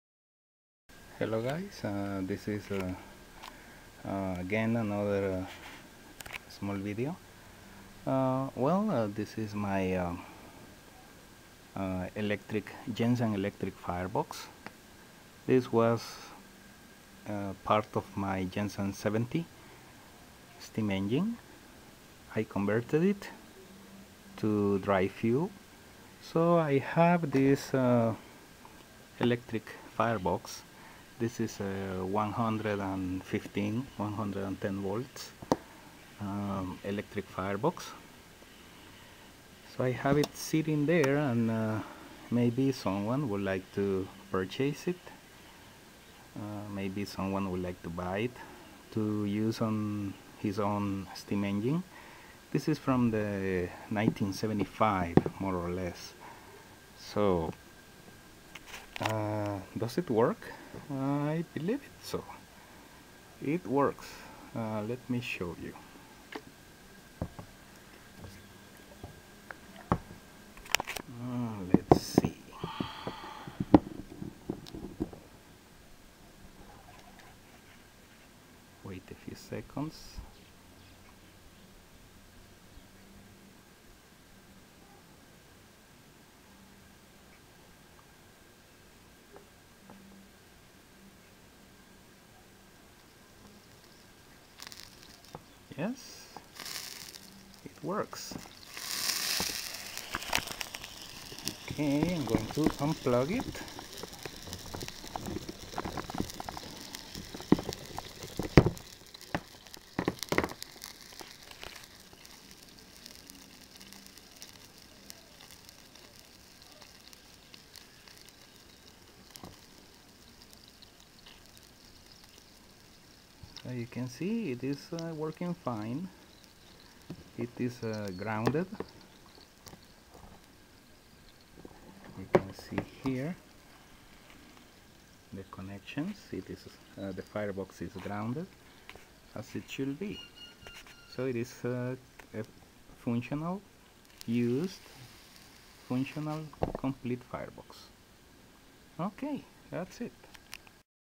Jensen Electric Firebox 125V - SOLD!
This Jensen Electric Firebox with good working heater was originally installed in my Jensen 70 Vintage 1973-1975. This Jensen 70 was converted to dry fuel fired.
I keep here this Electric firebox but is time for it to go.
SOLD On its way to a new home in Florida. !

Rob